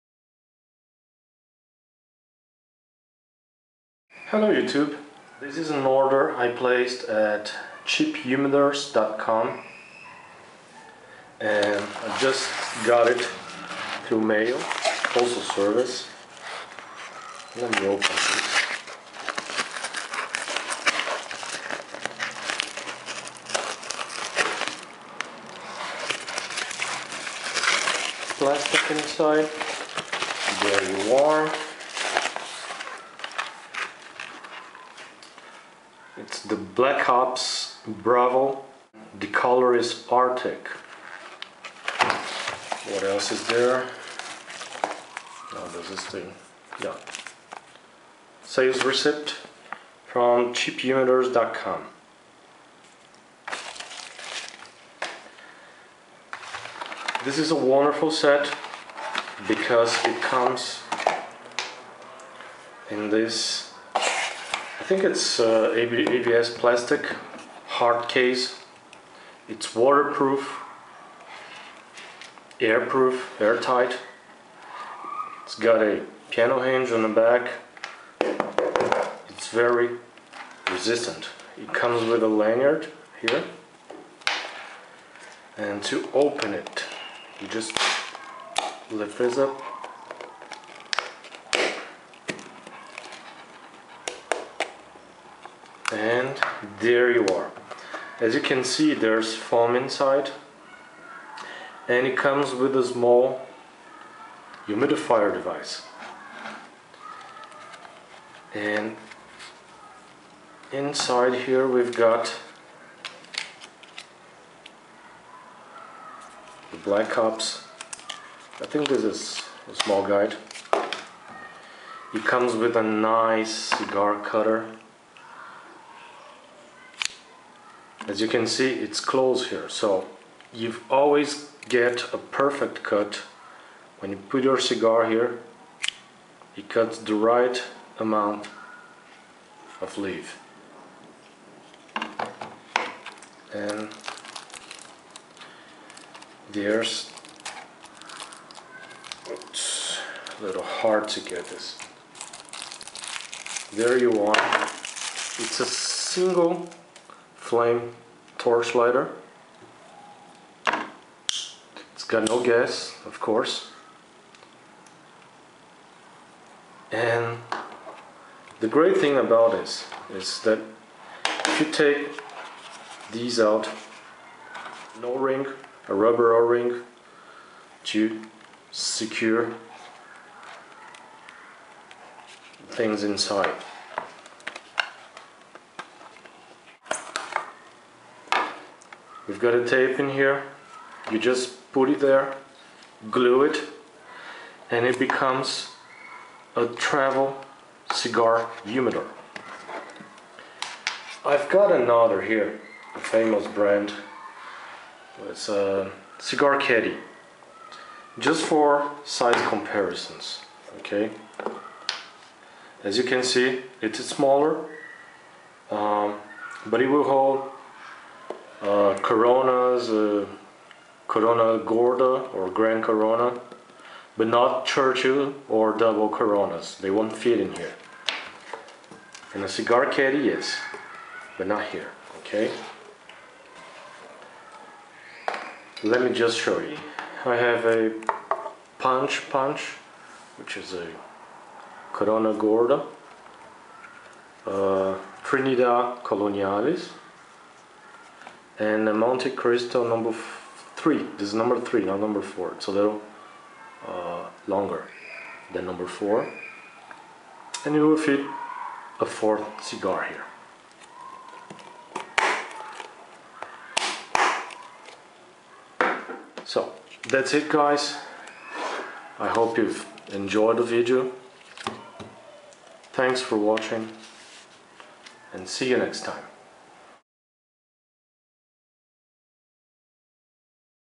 Black-Ops Cigar Set
This is un unboxing of a Black-Ops Cigar set. It comes with a double blade cutter, a single flame torch lighter and a plastic travel case with foam and a humidifier device inside. Take a look at: